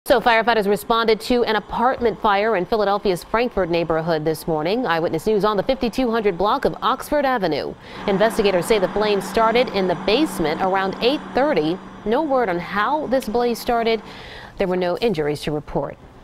Crews Battle Flames At Frankford Apartment Building
No one was injured.